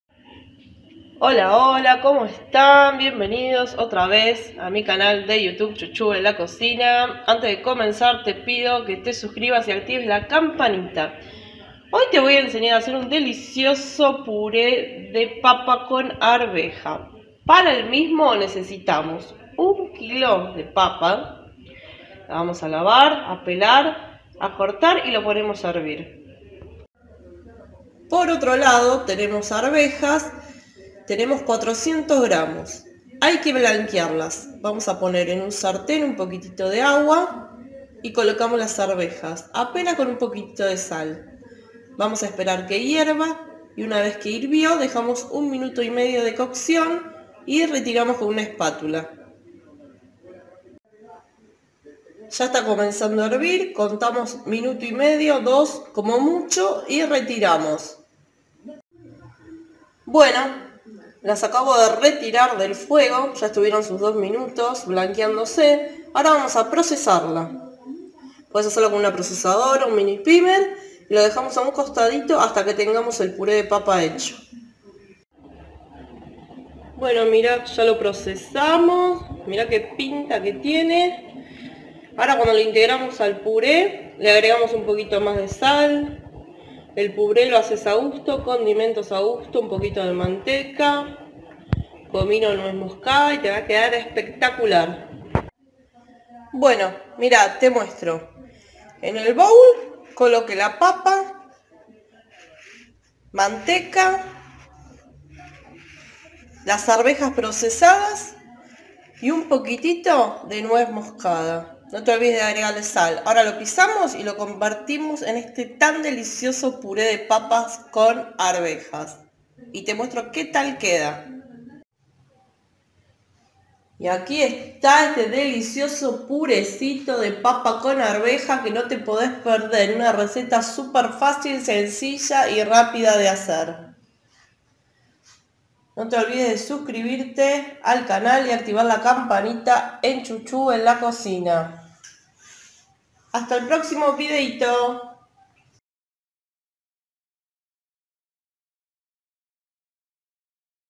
Puré De Papas Con Arvejas / CHUCHU EN LA COCINA
Delicioso Puré, ideal para que los peques se acostumbren a los colores a la hora de la comida!!